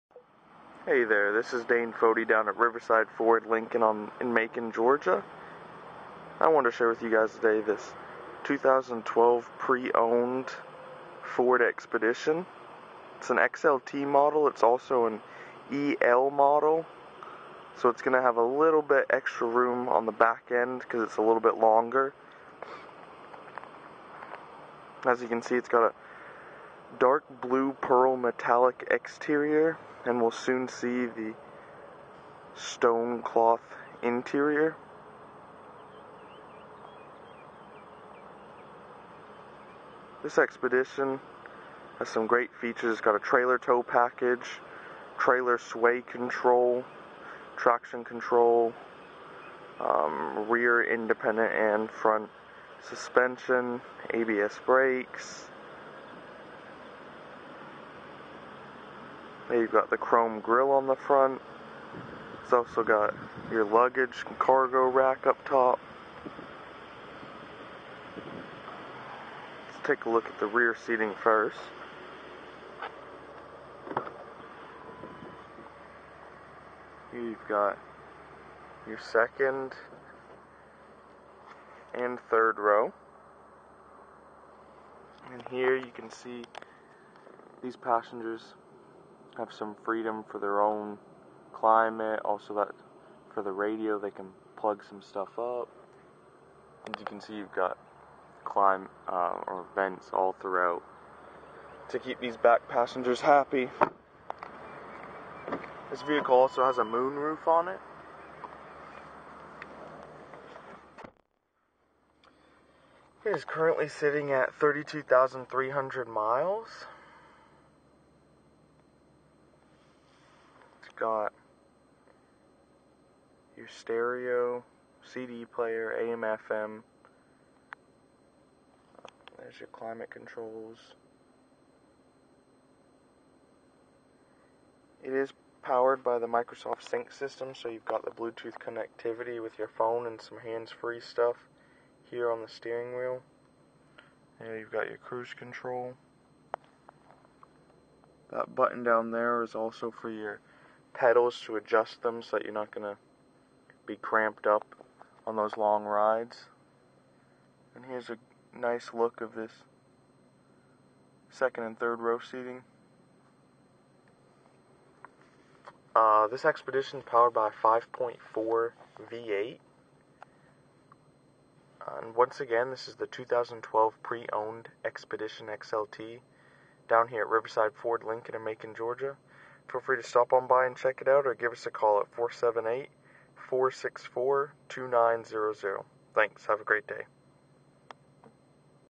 2012 Ford Expedition EL XLT B1221
Expedition EL XLT, Carfax one owner, Fully automatic headlights, Radio: Premium Sound System w/Single-CD/MP3 Player, SIRIUS Satellite Radio, and Steering wheel mounted audio controls. If you've been thirsting for the perfect 2012 Ford Expedition EL, well stop your search right here. This is the ideal SUV that is sure to fit your needs. Have one less thing on your mind with this trouble-free Expedition EL.